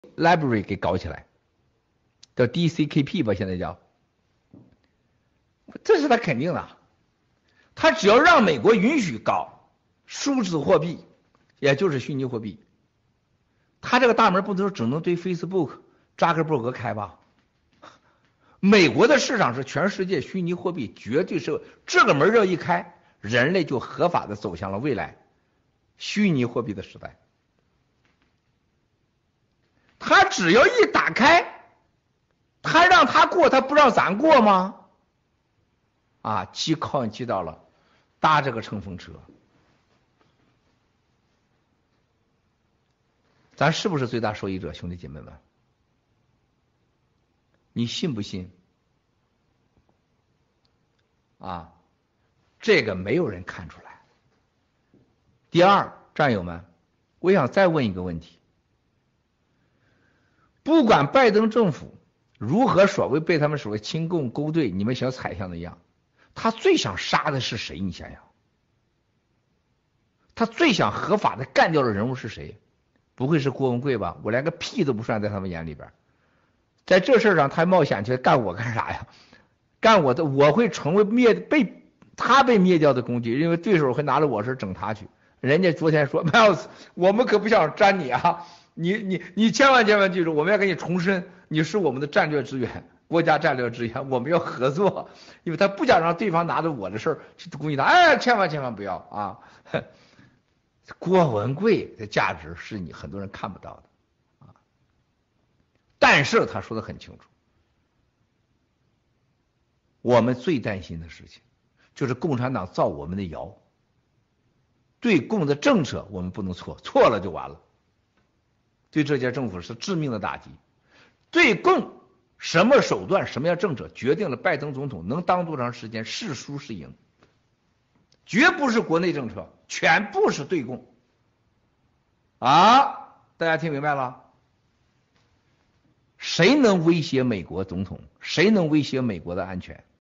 2021年1月25日
拜登的上台带给新中国联邦的好处是什么？